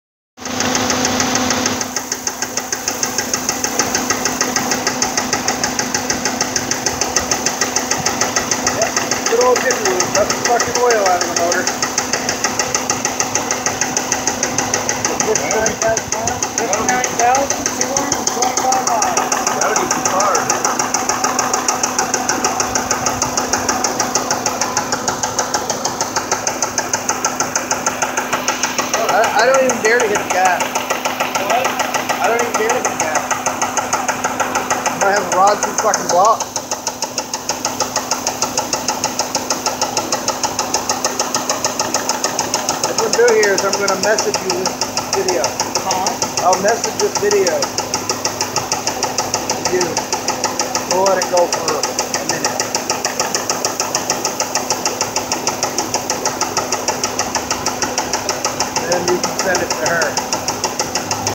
Jiffy Lube Oil change....Forgot the oil...
As the title says. This car came in complaining of noise after they had a oil change at Jiffy Lube... the motor was junk and needed replacement.